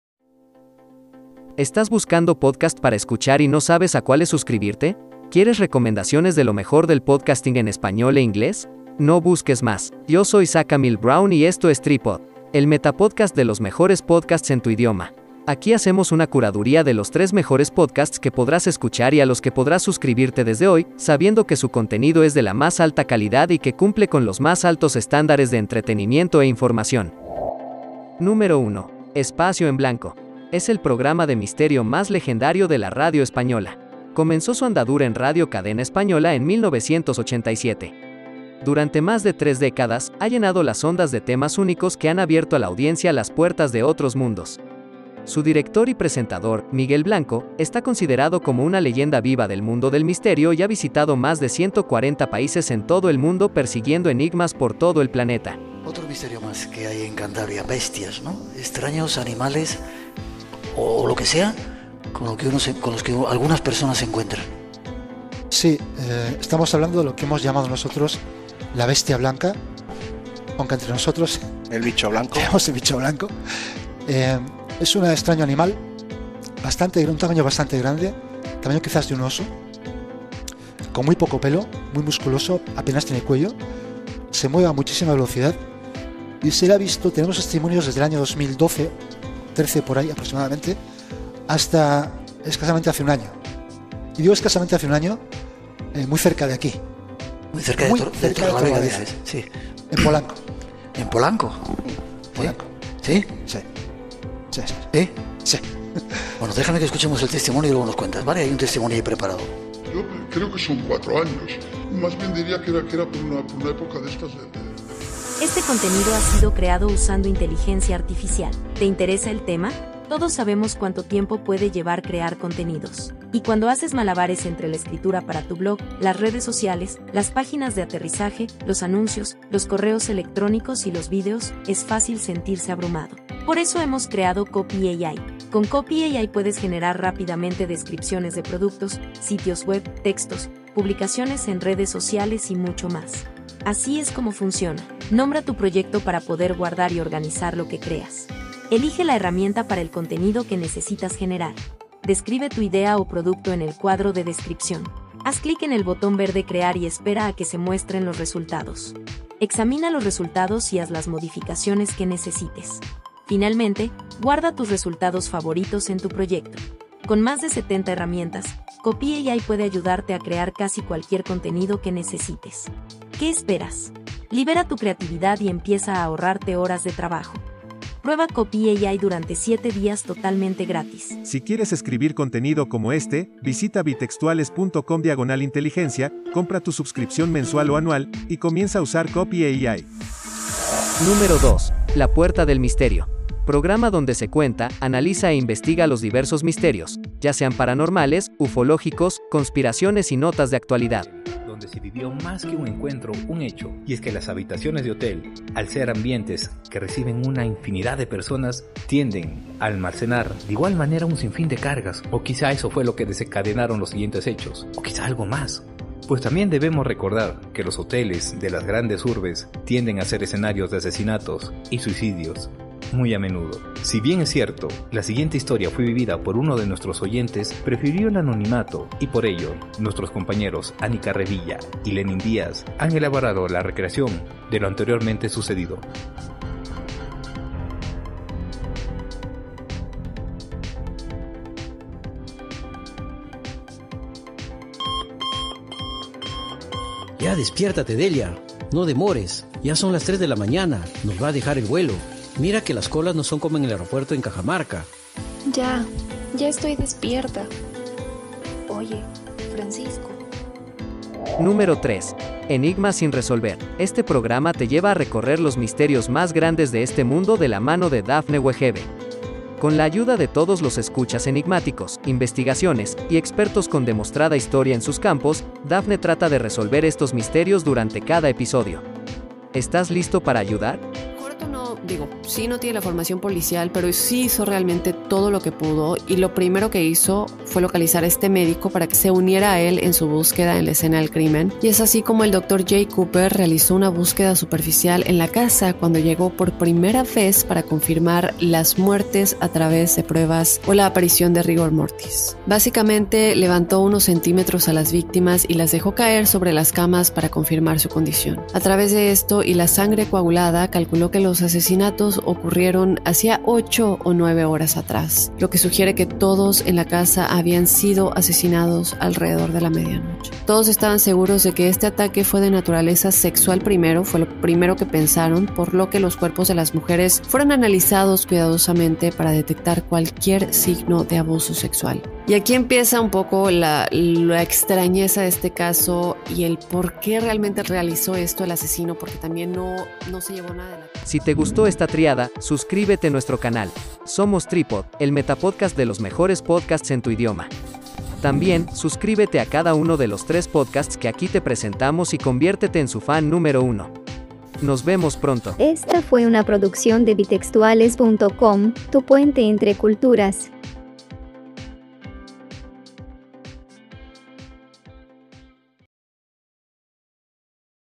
Los tres misteriosos
¿Quieres recomendaciones de lo mejor del podcasting en español e inglés? ¡No busques más!

Número uno: Espacio en blanco.

Es el programa de misterio más legendario de la radio española. Comenzó su andadura en Radio Cadena Española en 1987. Durante más de tres décadas, ha llenado las ondas de temas únicos que han abierto a la audiencia las puertas de otros mundos. Su director y presentador, Miguel Blanco, está considerado como una leyenda viva del mundo del misterio y ha visitado más de 140 países en todo el mundo persiguiendo enigmas por todo el planeta.

Número dos: La puerta del misterio.

Programa donde se cuenta, analiza e investiga los diversos misterios, ya sean paranormales, ufológicos, conspiraciones y notas de actualidad.

Número tres: Enigmas sin Resolver.

Este programa te lleva a recorrer los misterios más grandes de este mundo de la mano de Dafne Wejebe. Con la ayuda de todos los escuchas enigmáticos, investigaciones, y expertos con demostrada historia en sus campos, Dafne trata de resolver estos misterios durante cada episodio. ¿Estás listo para ayudar?

Somos Trípod, ¡el metapodcast de los mejores podcasts en tu idioma!  ¡Nos vemos pronto!

No tiene ningún costo extra para ti, sin embargo, nos ayuda a nosotros para seguir produciendo material como este.